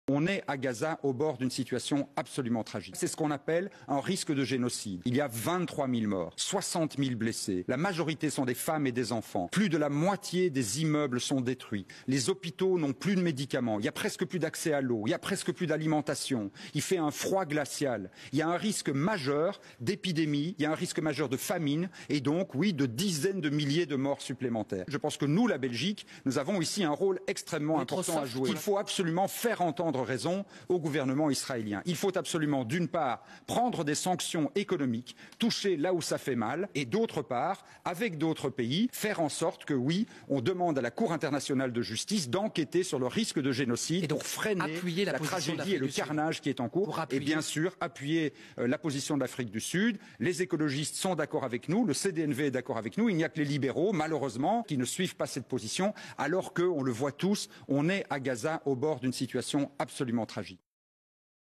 Paul Magnette maire de Charleroi: "Il faut prendre des sanctions économiques contre Israël"
Paul Magnette, président du PS belge et maire de Charleroi: "Il faut prendre des sanctions économiques contre Israël (...) et avec d'autres pays faire en sorte qu'on demande à la Cour Internationale d'enquêter sur le risque de génocide pour freiner la tragédie et le carnage en cours"